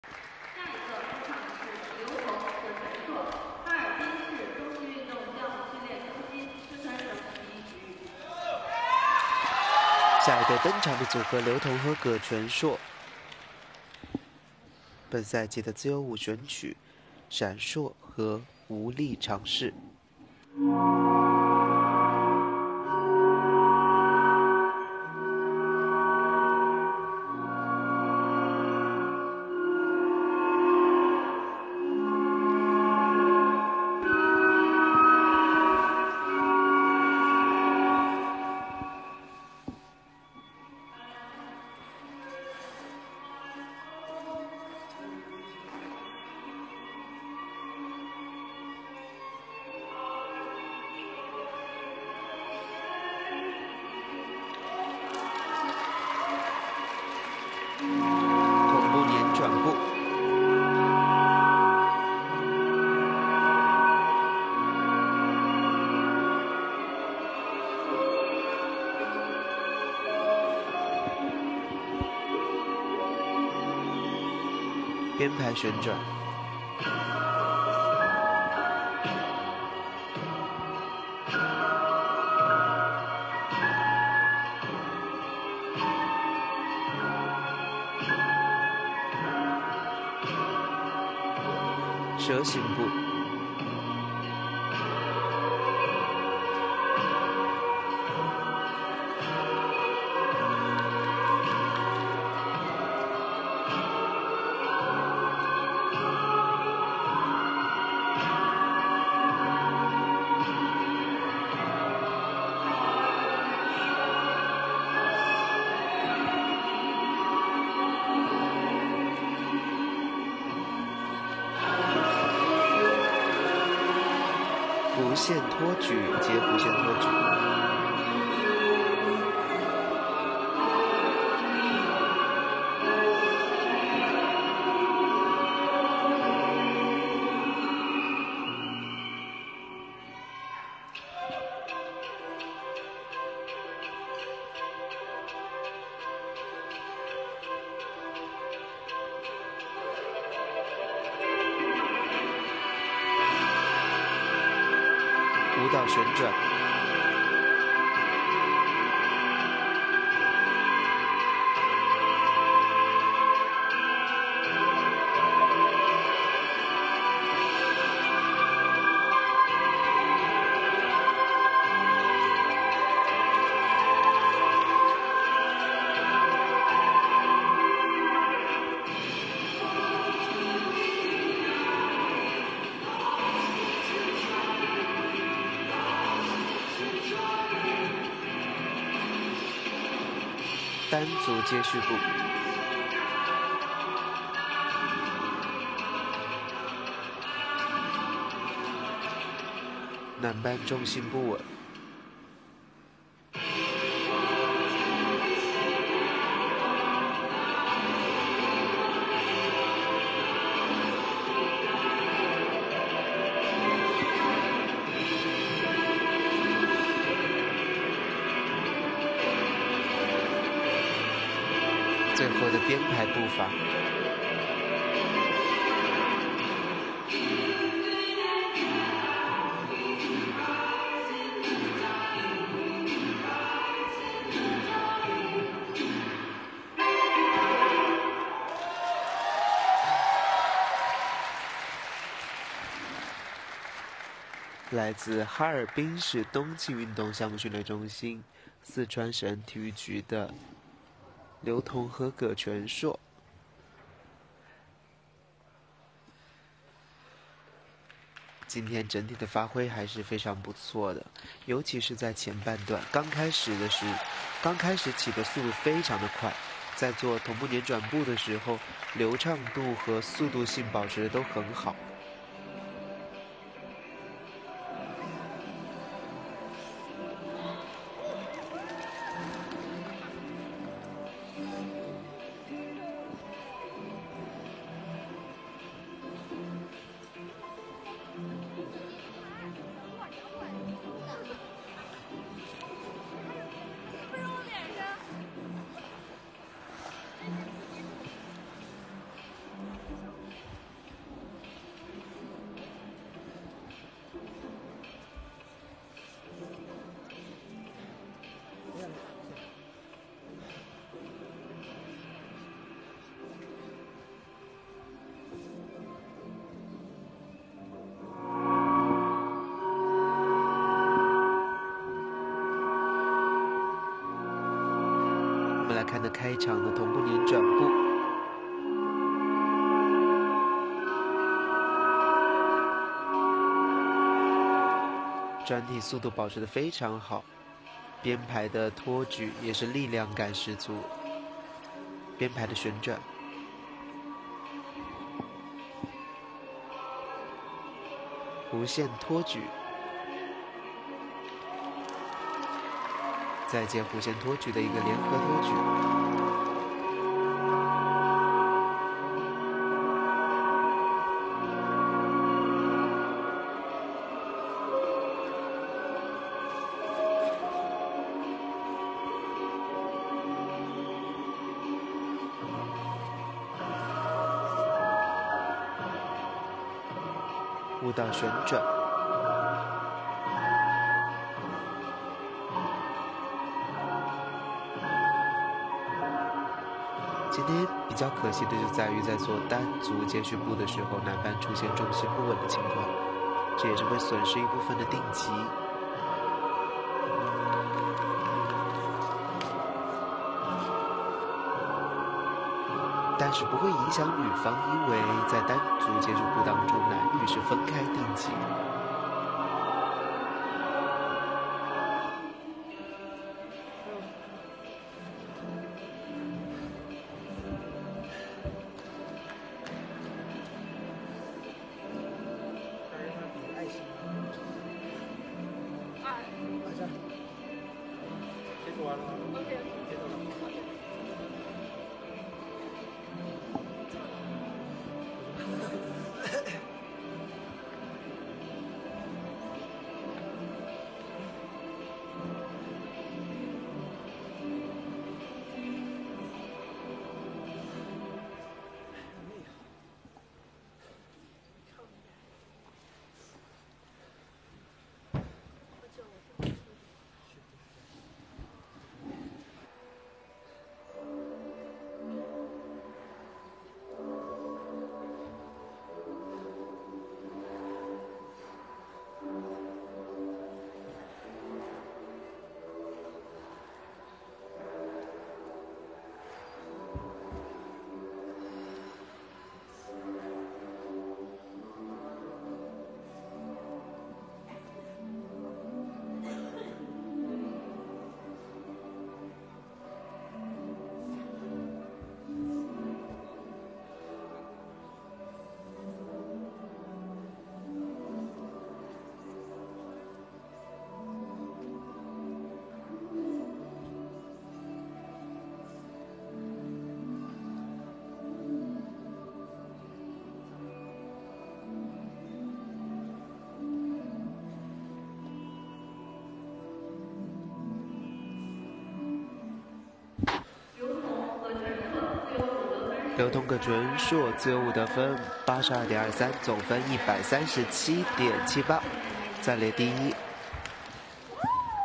Tong LIU / Quanshuo GE｜2023Chinese National Junior Figure Skating Championships Ice Dance Free Dance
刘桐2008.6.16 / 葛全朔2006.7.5

Looking forward to the YOG, LIU / GE's debut in the international competition!!!